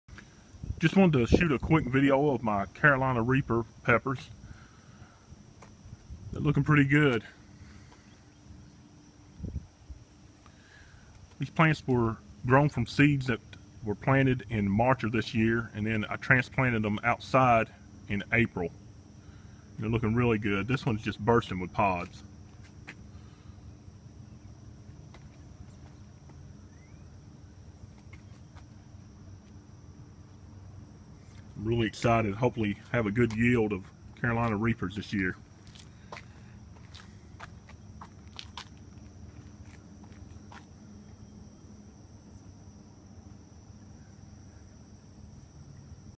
Carolina Reaper pepper plants in pots. July 21, 2015.
A short video of my Carolina Reaper hot pepper plants.  Germinated in March and transplanted outside in April.  I used Miracle-Gro potting mix to plant them. Looking good so far.